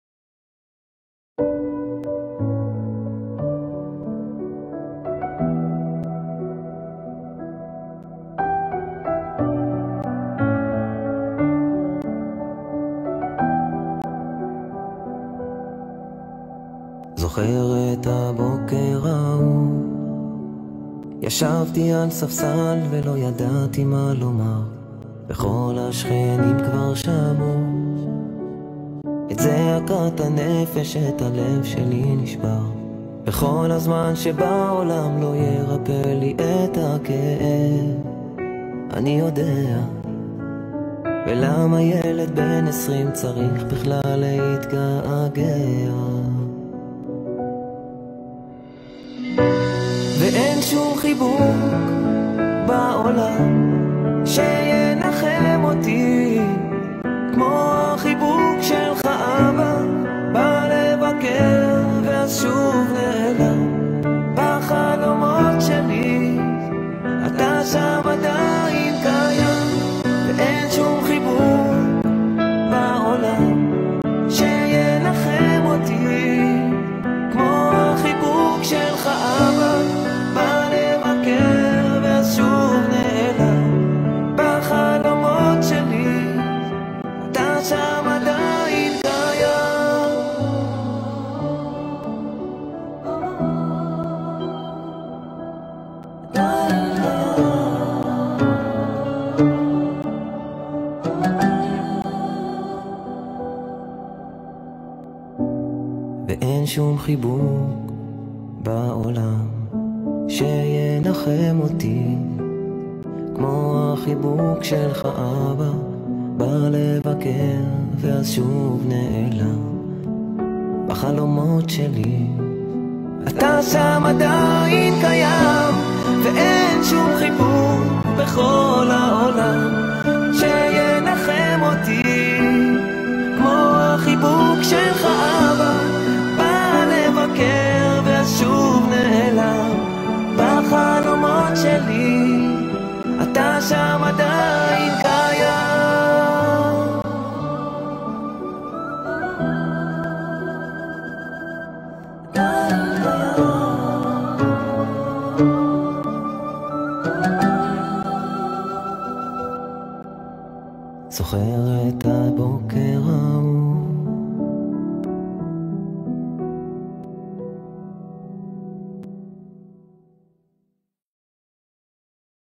Israeli comedian, Etzion Musai, sings powerfully emotional song for his fallen IDF soldier father
Wow I totally missed this incredibly powerful and emotional song that was written by my wife's cousin, Etzion Musai, who wrote it for his late father who fell while serving in the IDF. Etzion was 20 years old when his dad died.

He released it on Yom HaZikaron (memorial day) and the picture is of him at his father's grave in the military cemetery in our city. Etzion wrote the lyrics to many of Eyal Golan's songs and other famous artists too.

This song was produced by Dor Brin.  I added English subtitles to it.

May the memory of his father be for a blessing and to you Etzion, may your pain heal brother!